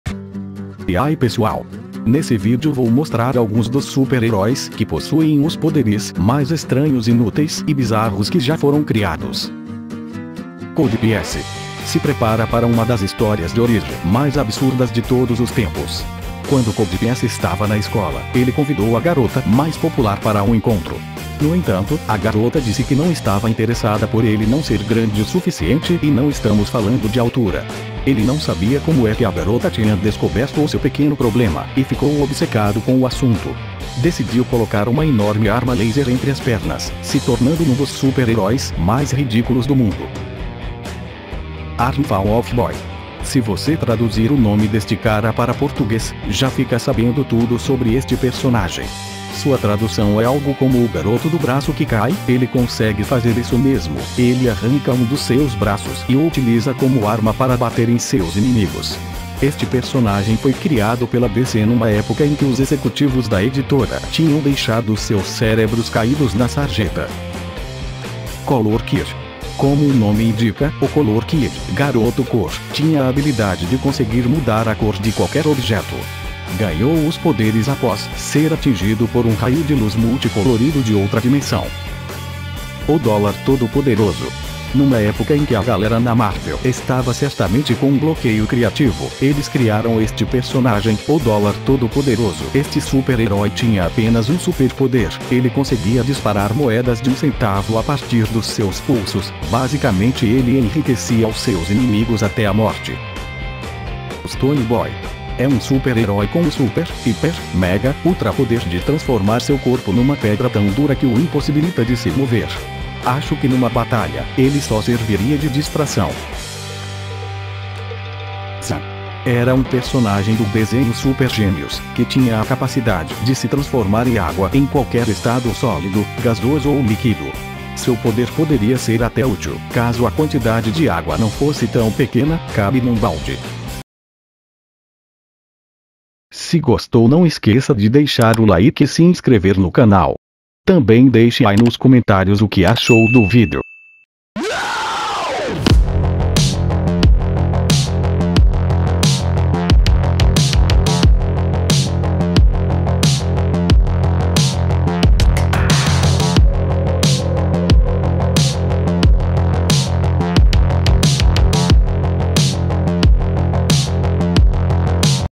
OS SUPER PODERES MAIS BIZARROS E INÚTEIS JÁ CRIADOS
Nesse video irei falar um pouco sobre  herois e vilões com super poderes  bizarros e inuteis que possivelmente você não sabia que existia.